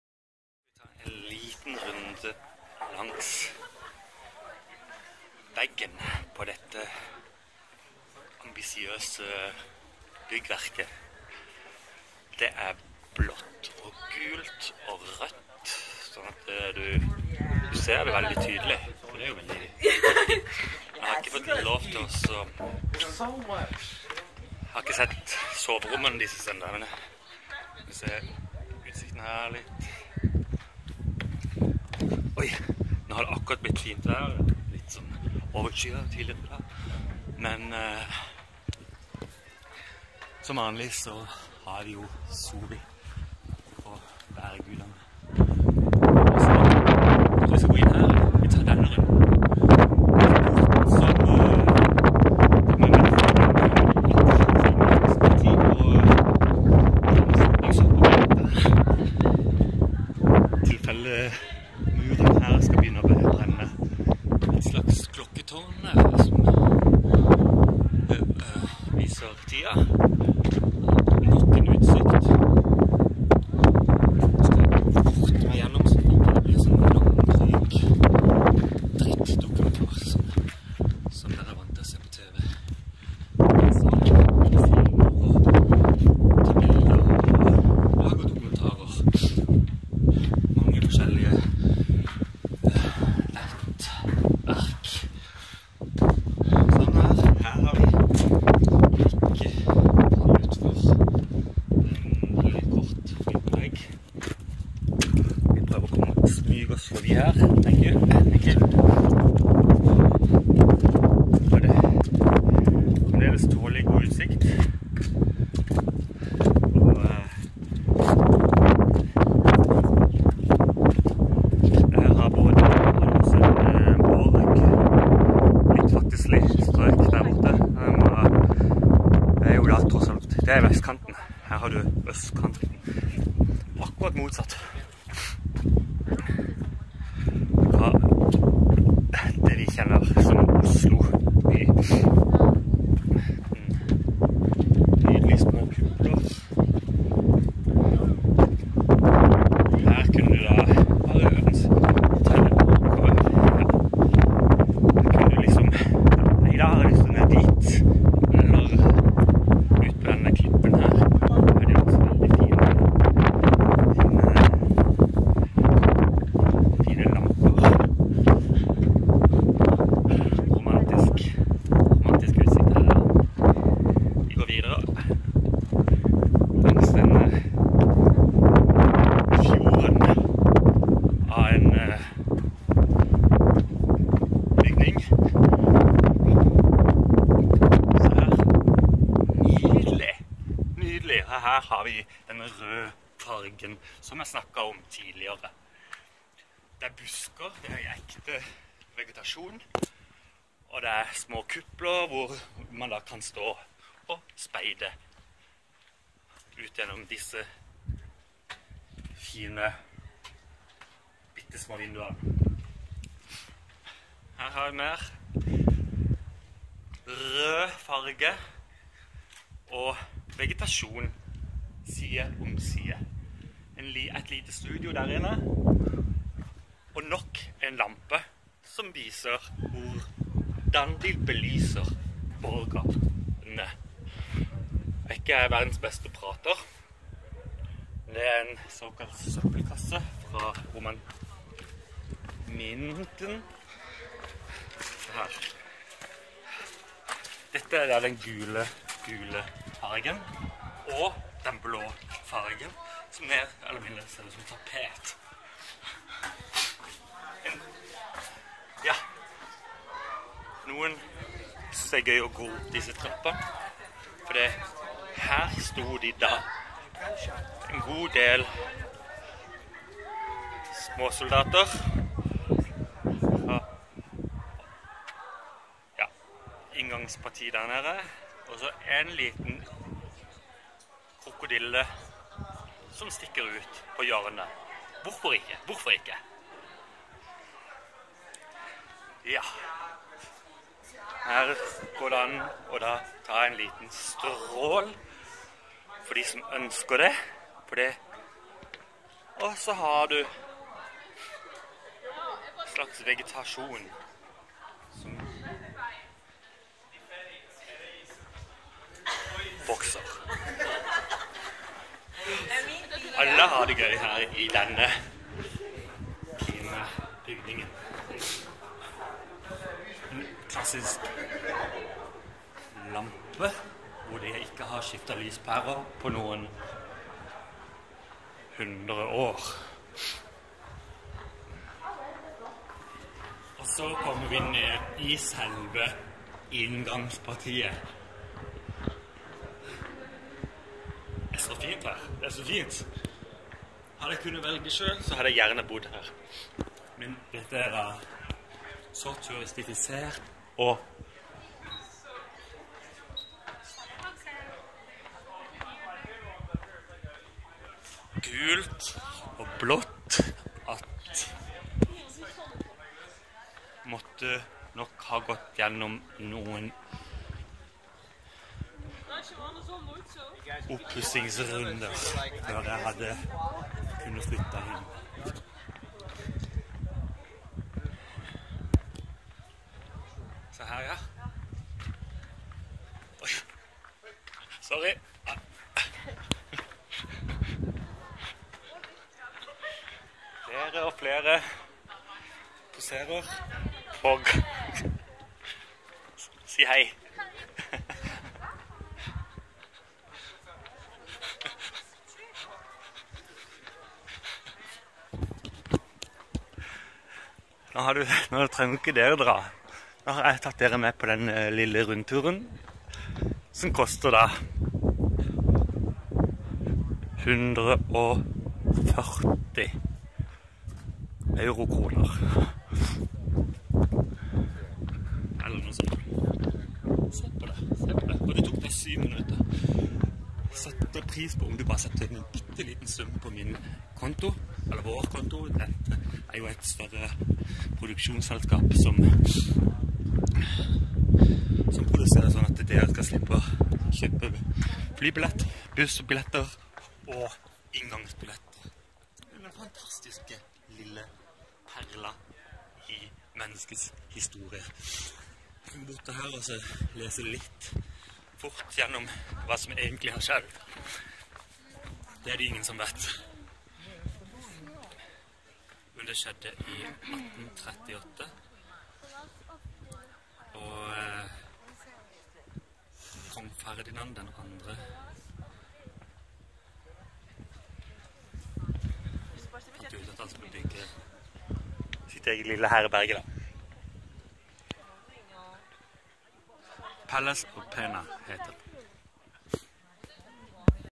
The Palace of Pena
Documentary (true) about a yellow and blue an green building.